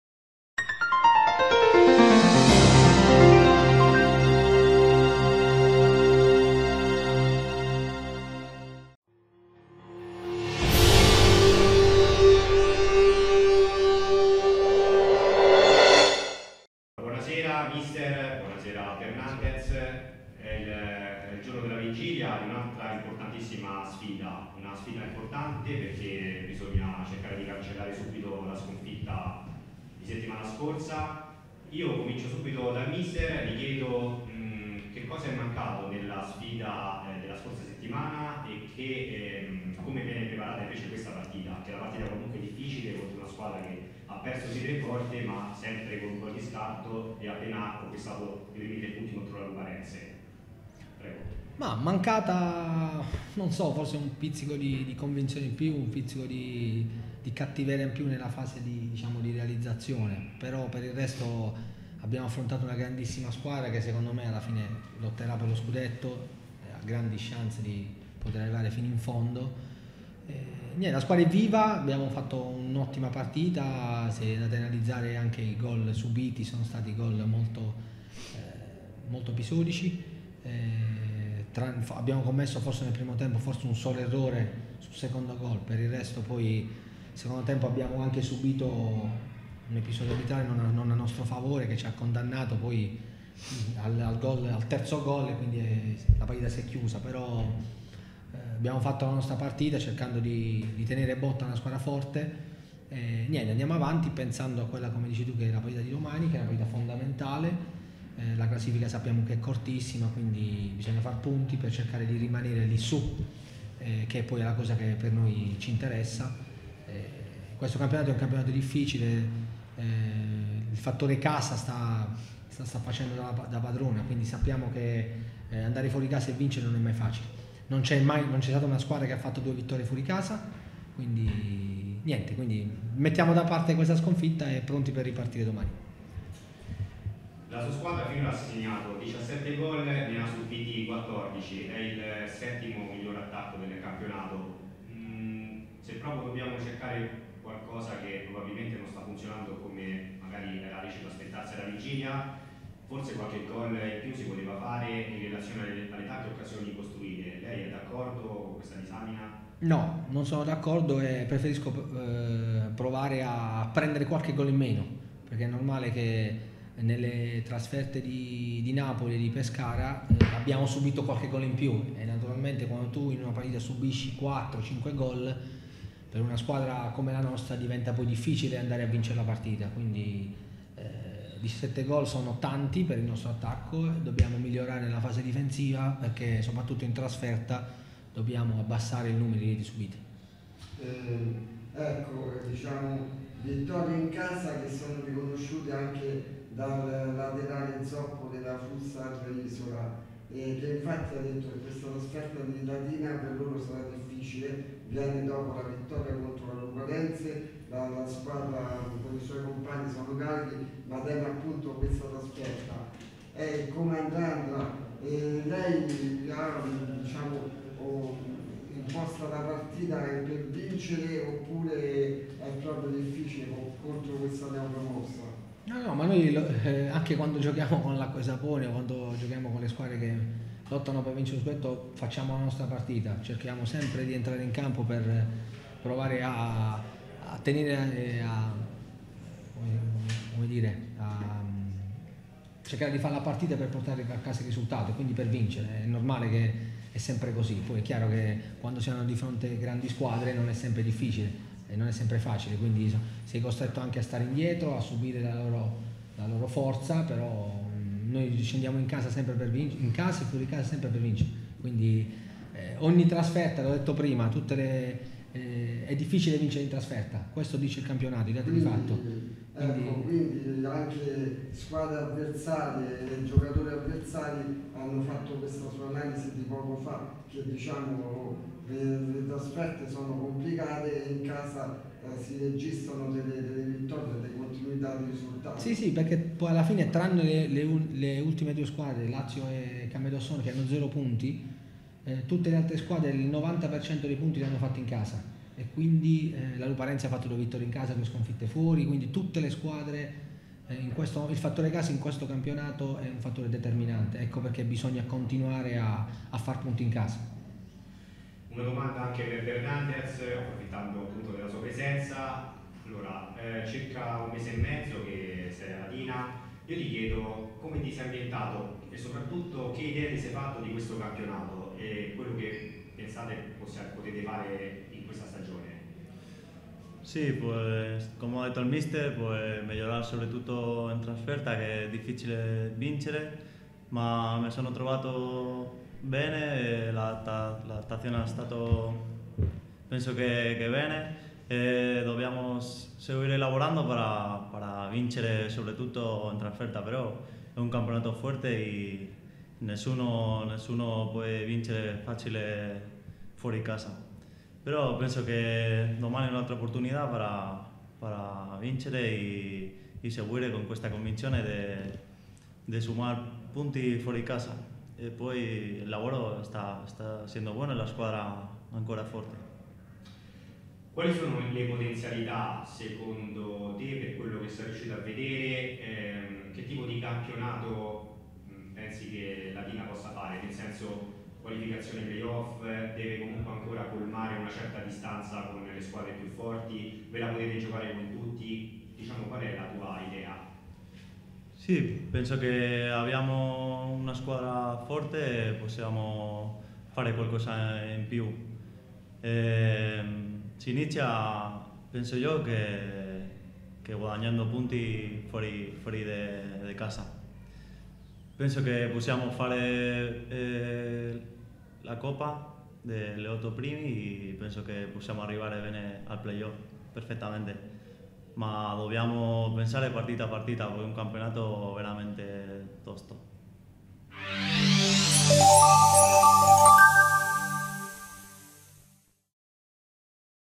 Serie A: Axed Group Latina - conferenza stampa pre gara
Sono intervenuti Mister Basile e Bernardez